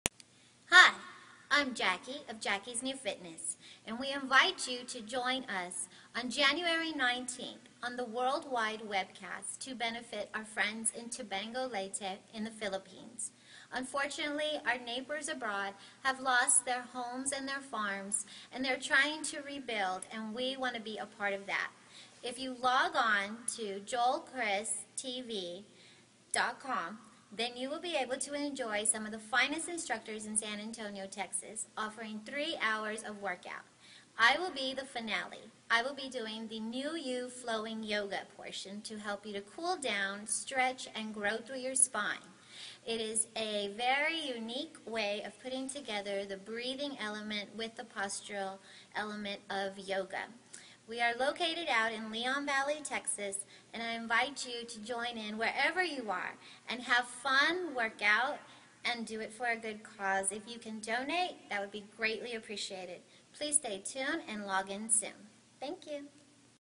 Personal Invitation from Jacqi Hill of Jacqi's NU U FITNESS
THANK YOU Jacqi Hill for VOLUNTEERING your Time, Talent. Treasure & Team to our JANUARY 19TH WORKOUTHON (Worldwide Workout FUNdraising Telethon) to Help Tabango Leyte, Philippines REBUILD!

Get FIT with Nu U Pilates & Yoga Flow by Jacqi's Nu Fitness and JOIN US!

(Please "LIKE" & "SHARE" this photo. Thank you!  )